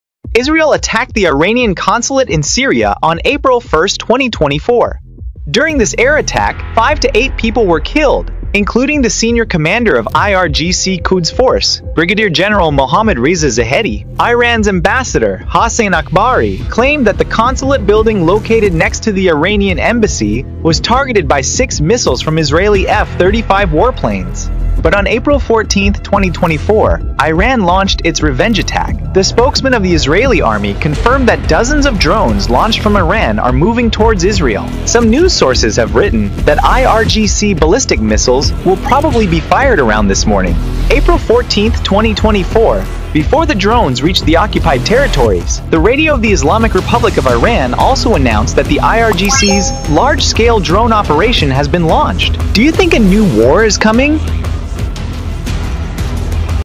Why did Iran launch missile attacks on Israel?
Israel attacked the Iranian consulate in Syria on April 1, 2024.  During this air attack, five to eight people were killed, including the senior commander of IRGC Quds Force, Brigadier General Mohammad Reza Zahedi.

 Iran's ambassador, Hossein Akbari, claimed that the consulate building located next to the Iranian embassy was targeted by six missiles from Israeli F-35 warplanes.

 But on April 14, 2024, Iran launched its revenge attack

 The spokesman of the Israeli army confirmed that dozens of drones launched from Iran are moving towards Israel.

  Some news sources have written that IRGC ballistic missiles will probably be fired around this morning, April 14, 2024, before the drones reach the occupied territories.

 The Radio of the Islamic Republic of Iran also announced that the IRGC's large-scale drone operation has been launched.

 Do you think a new war is coming?